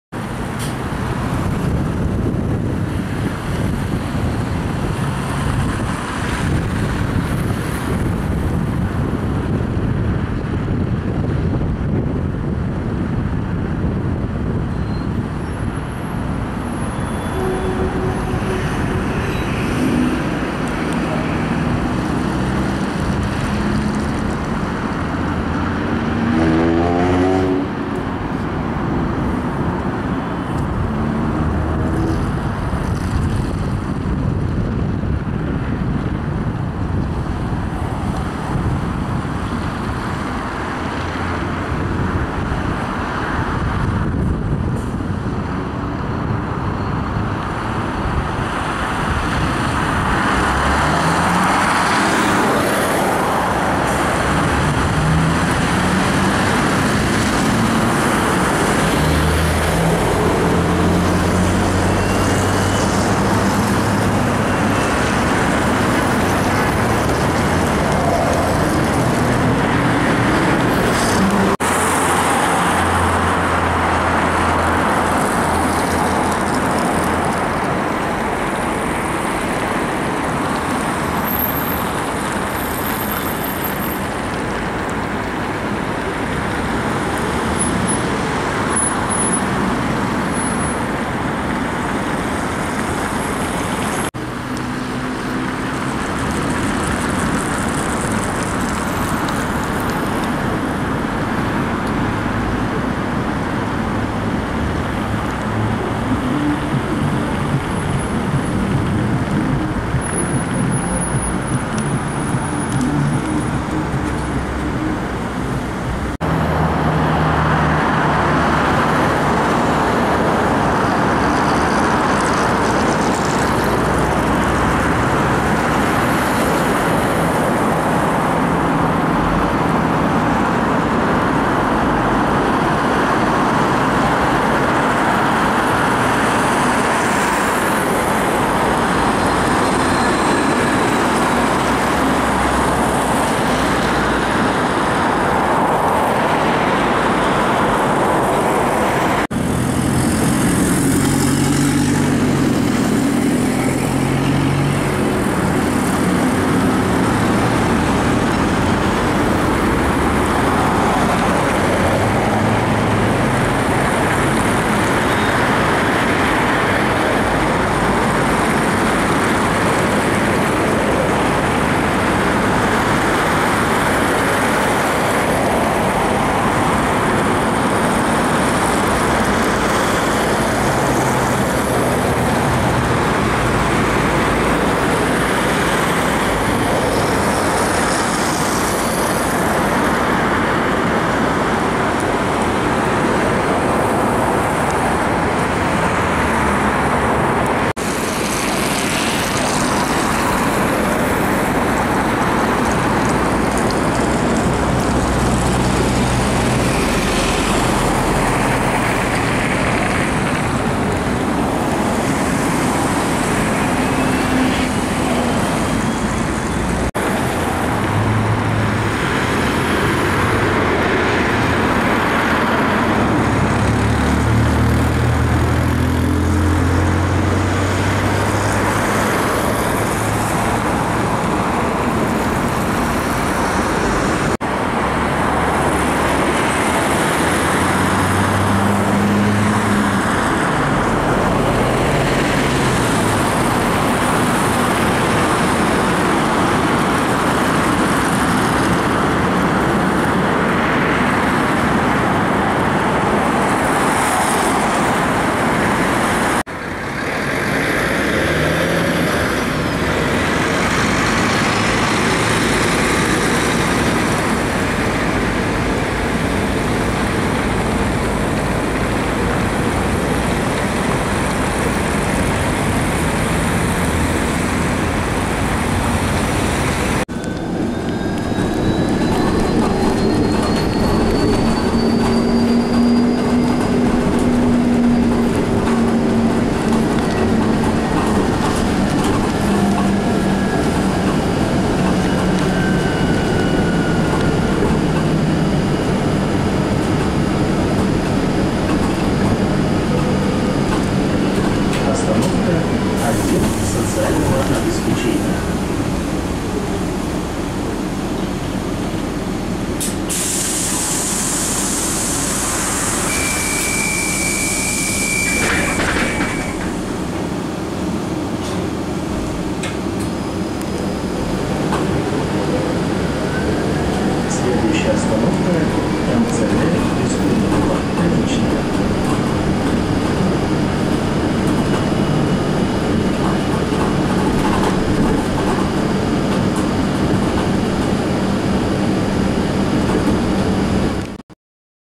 Moscow Buses 2021 - 29.03.2021
Moscow Buses 2021 - Video with Moscow buses. There are already many electric buses in Moscow. And now there are no trolleybuses.

Видео про автобусы Москвы. Это тоже старая тема моего канала. В Москве теперь много электробусов и нет троллейбусов.